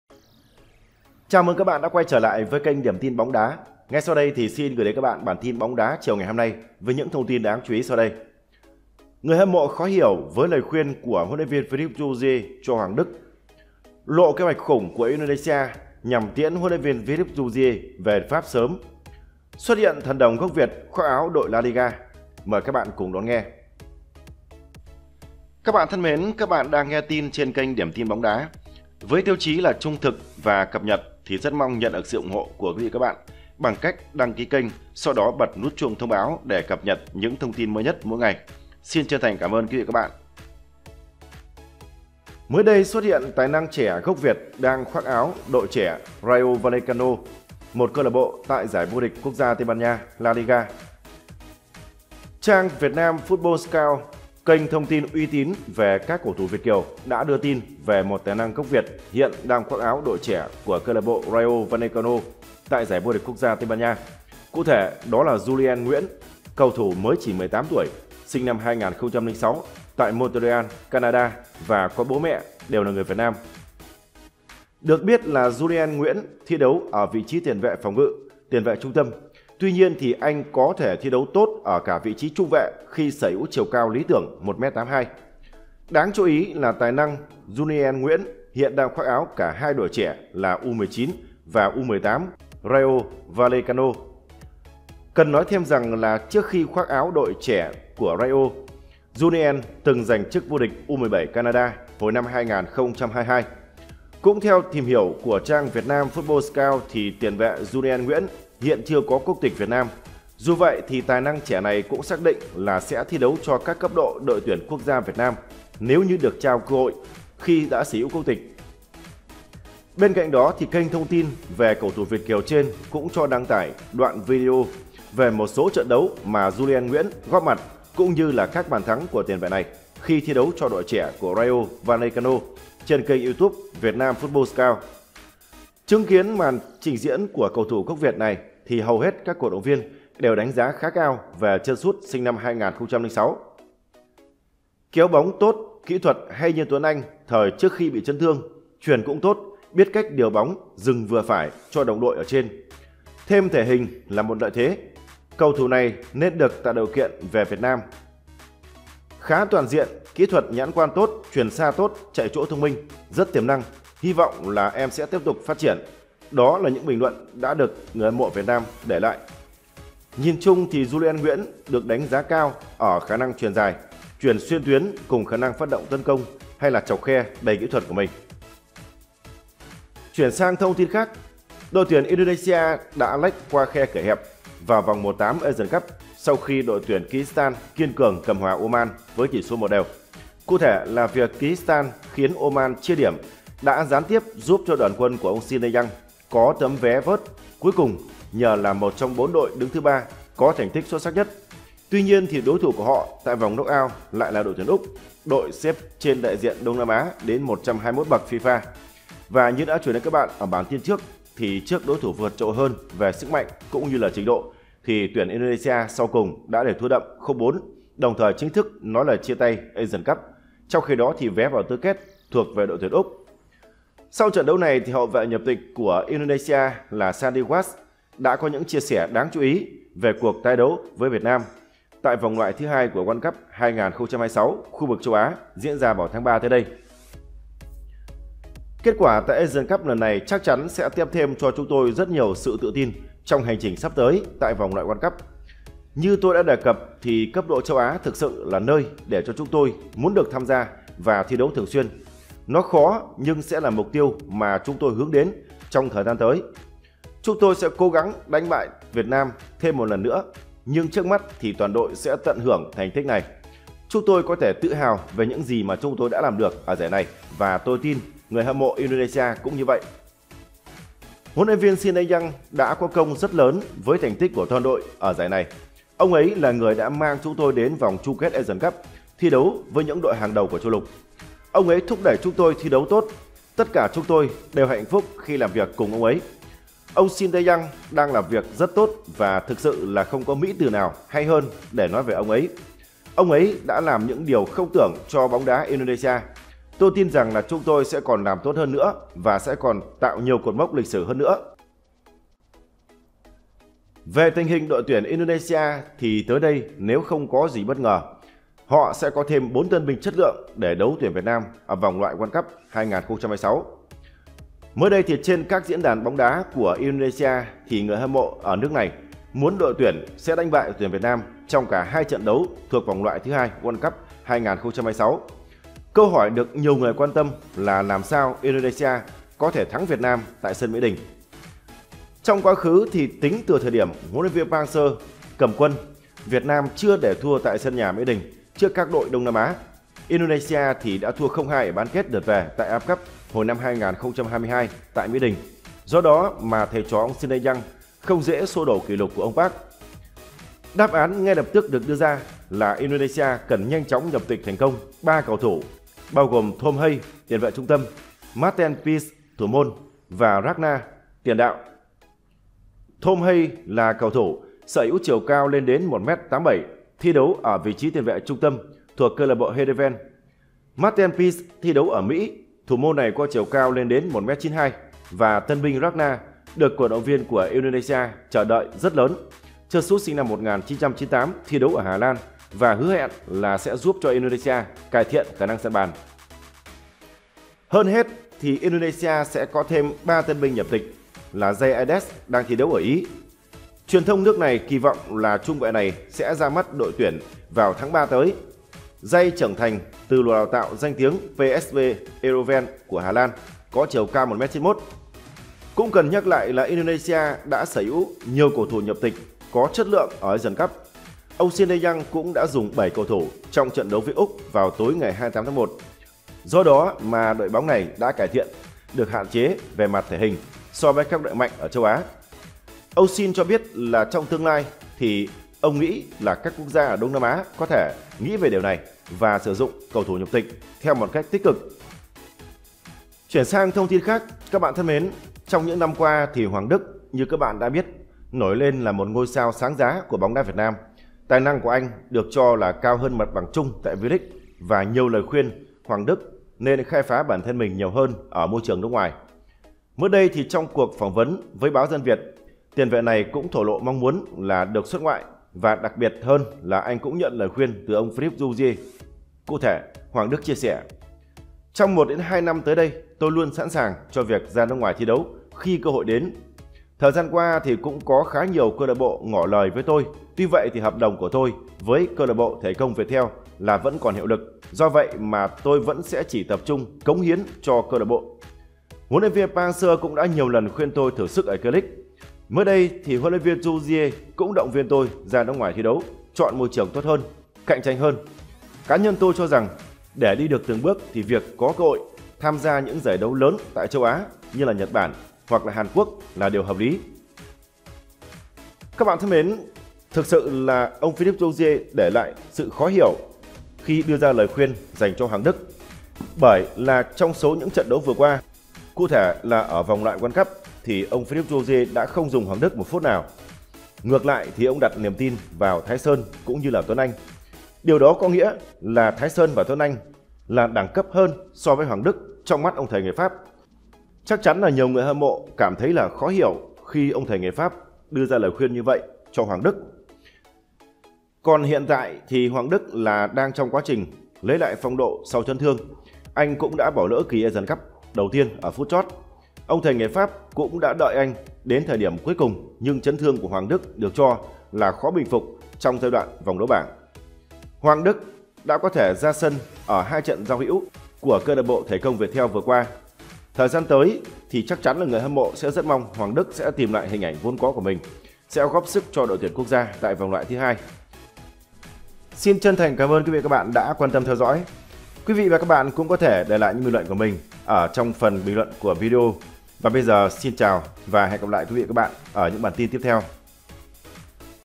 🔴 TIN NÓNG 29/1: INDONESIA LỘ KẾ HOẠCH TIỄN HLV TROUSSIER VỀ PHÁP SỚM HƠN DỰ KIẾN
🔴 HAGL THẮNG TRẬN ĐẦU THỜI HẬU KIATISAK, HLV TROUSSIER SẮP TAN MỘNG VÌ ĐI VÀO VẾT XE ĐỔ